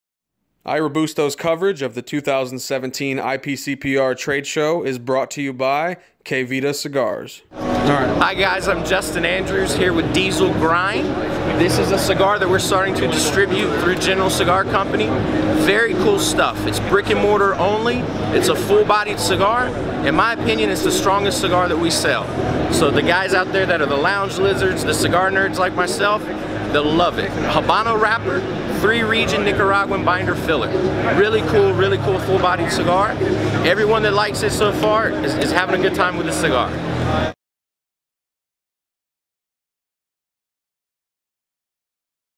IPCPR 2017 | Diesel Cigars
We get a first hand look at the DIesel Grind from Justin Andrews - a new cigar to the General Cigar portfolio.